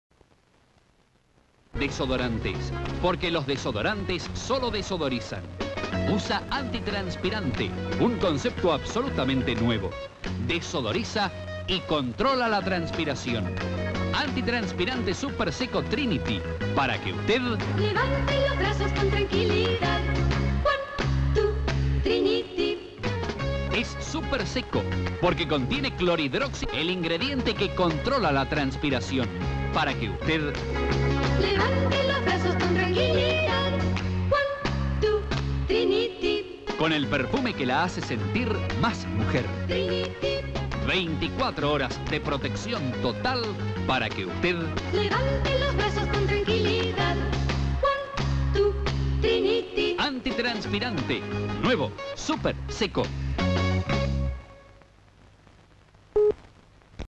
Trinity Twist, publicidad HD
Argentina, circa 1979
Protagonista: Nora Elena Ott de Zietlow
Locución: Leopoldo Costa